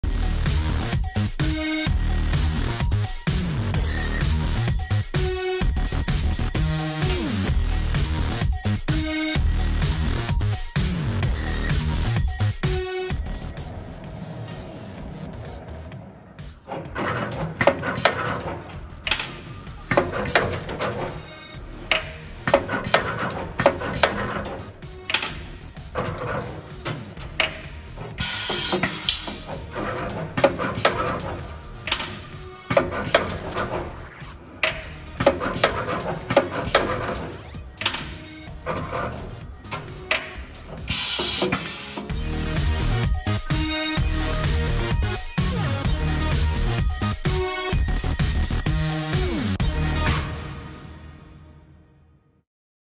Drahtbiegemaschine DB-6
Drahtbiegemaschine zum biegen von Draht von 2 bis 6 mm. 3 Achsen plus schneiden. Von der Firma Gelber-Bieger

Wire Bending machine up to 6 mm